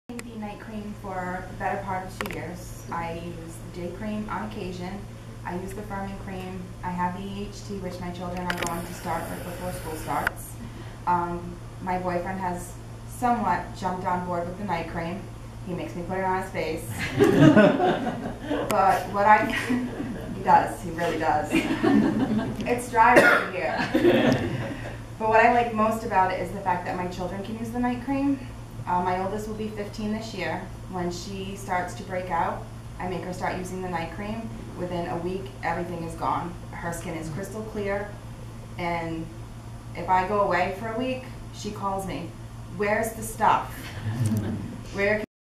Testimonial - Samantha Barth
How Nerium's Night, Day and Firming Cream helps her. Her boyfriend uses it for dry skin, and her teen daughter uses it to completely clear up acne breakouts within a week.